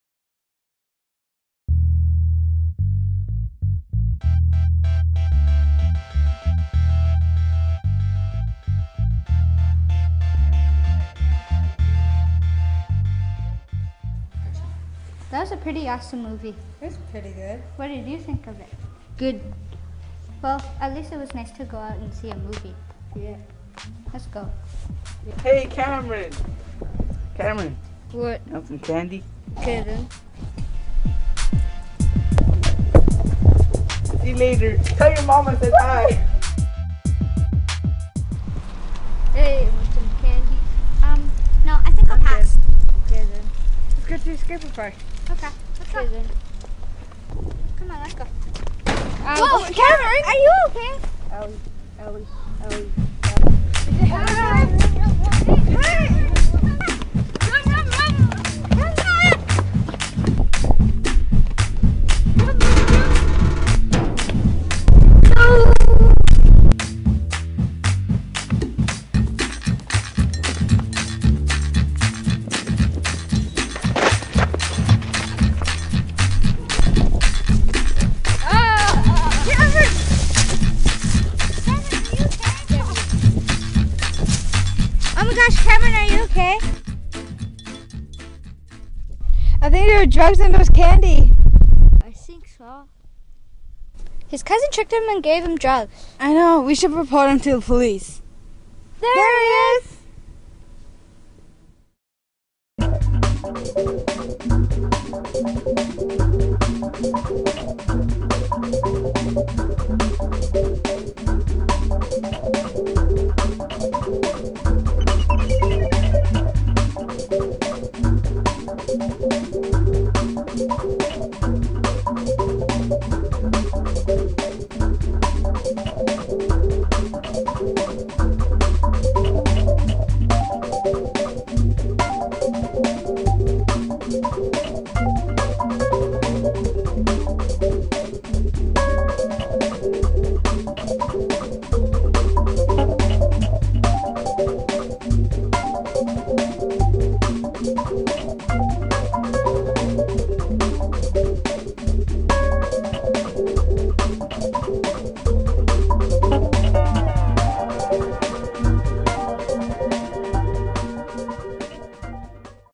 Candy
Inspired by a true story, a young child is tricked into thinking drugs are candy.